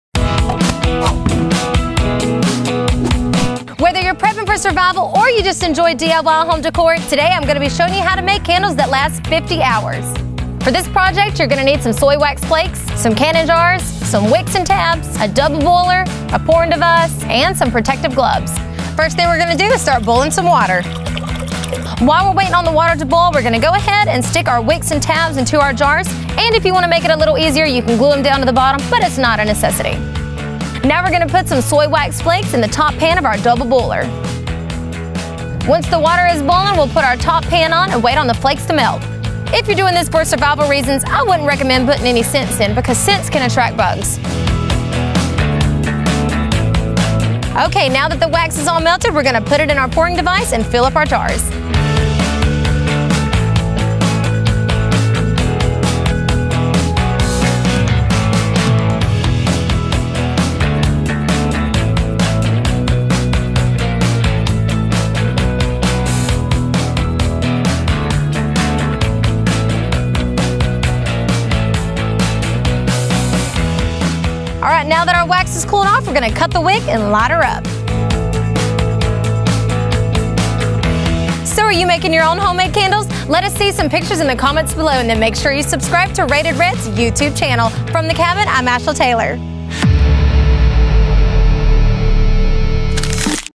How to Make Your Own 50-Hour Candles
For camping, power outages and/or the impending apocalypse.
Rated Red's Ashla Taylor shows you how to make your own 50-hour candles. Just don't make them scented if you're going to use them when camping!
Check this out!!!